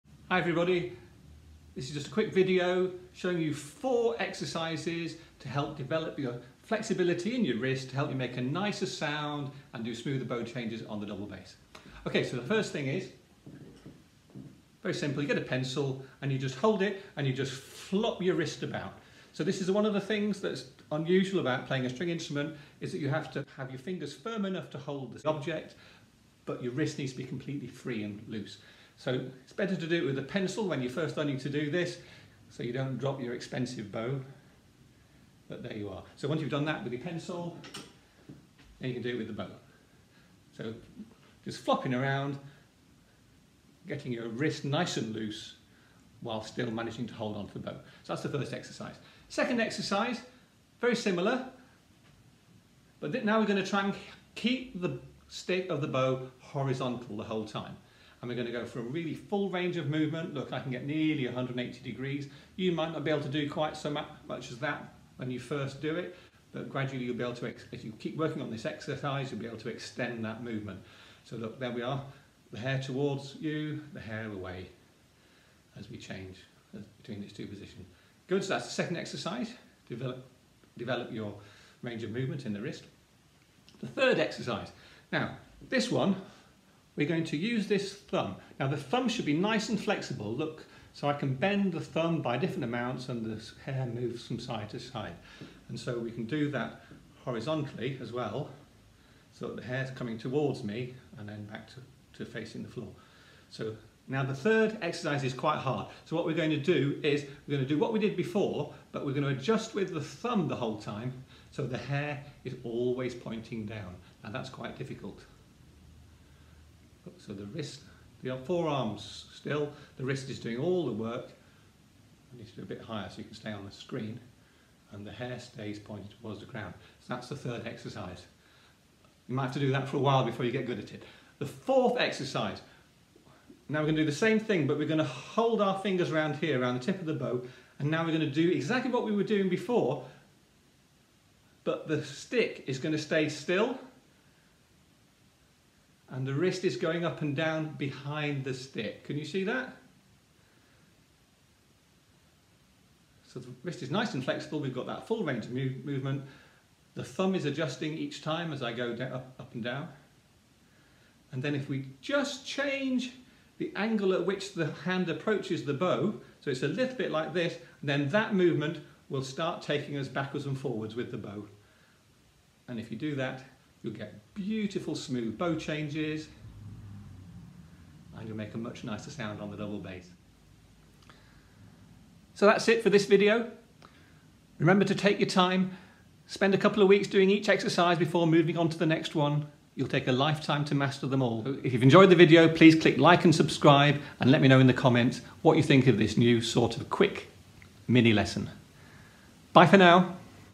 Four Exercises to improve bowing on the double bass
Chris West demonstrates four exercises designed to improve the flexibility of your wrist when bowing the double bass.